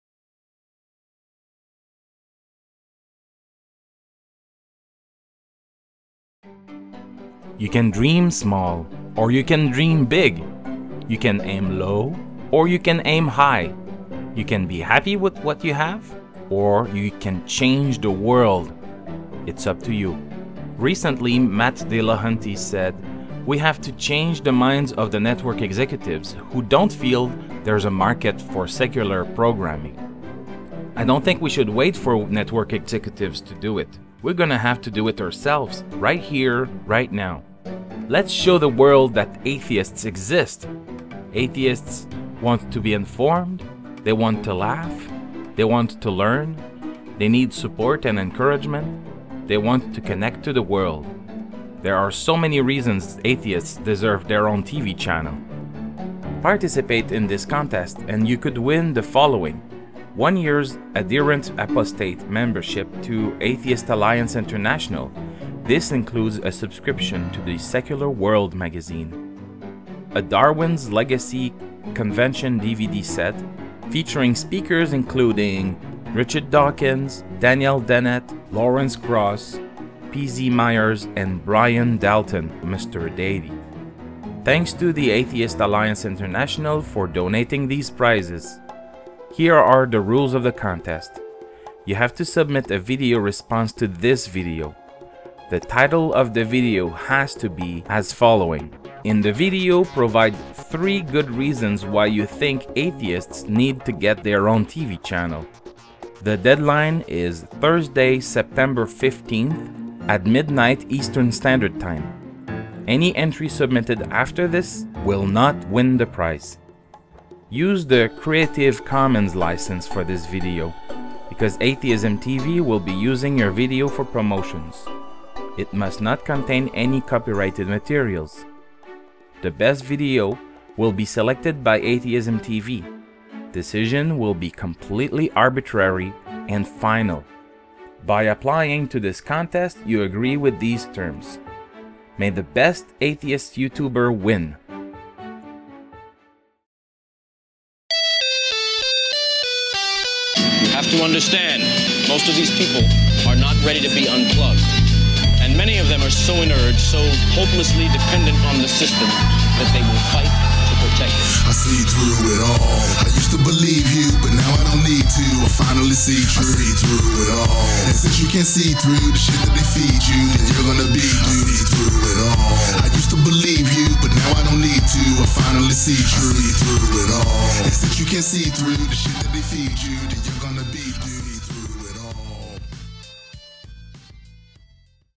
Contest: Top 3 reasons why Atheists deserve their own TV channel!
Thanks to Atheist Alliance International for donating the prizes.

You could win the following:
1- One year's "Ardent Apostate" membership to Atheist Alliance International. This includes a subscription to the Secular World magazine.
2- A Darwin's Legacy convention DVD set featuring speakers including Richard Dawkins, Daniel Dennett, Lawrence Krauss, PZ Myers and Brian Dalton (Mr Deity),

Rules of the contest:
1. Submit a video response to THIS video.
2. The title of the video has to be "Top 3 reasons why Atheists deserve their own TV channel!" In the video provide three good reasons why you think Atheists need to get their own TV channel.
3. The deadline is Thursday September 15th at midnight Eastern Standard Time. Any entry submitted after this will not win the prize.
4. Use the "Creative Commons" license for this video. It must not contain any copyrighted materials.
5. The best video will be selected by AtheismTV. Decision will be completely arbitrary and final.

By applying to the contest you agree with these terms.
May the best Atheist youtuber win!!!